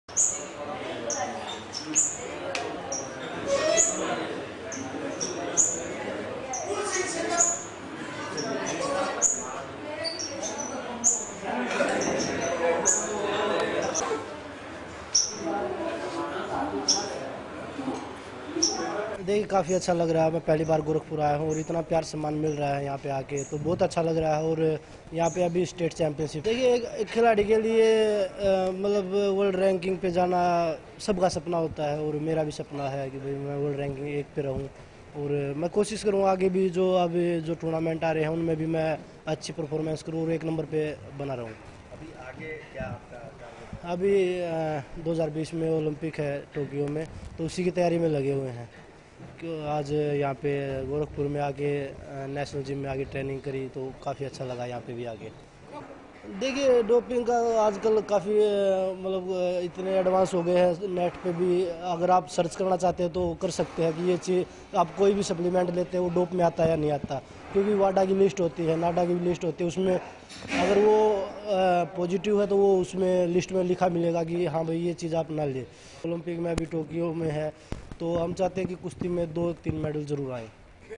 रेसलर पुनिया की युवाओं को नसीहत, स्टेरॉयड युक्त दवाओं का न करें सेवन...
गोरखपुर: गोल्ड मेडलिस्ट व अर्जुन पुरस्कार से सम्मानित रेसलर बजरंग पुनिया गोरखपुर में आयोजित होने वाली स्टेट रेसलिंग चैंपियनशिप में बतौर मुख्य अतिथि के रूप में सम्मलित होने आए. जहां उन्होंने रेसलिंग की दुनिया में युवाओं के बढ़ते रुझान और डोपिंग के आरोपों से बचने का रास्ता बताया.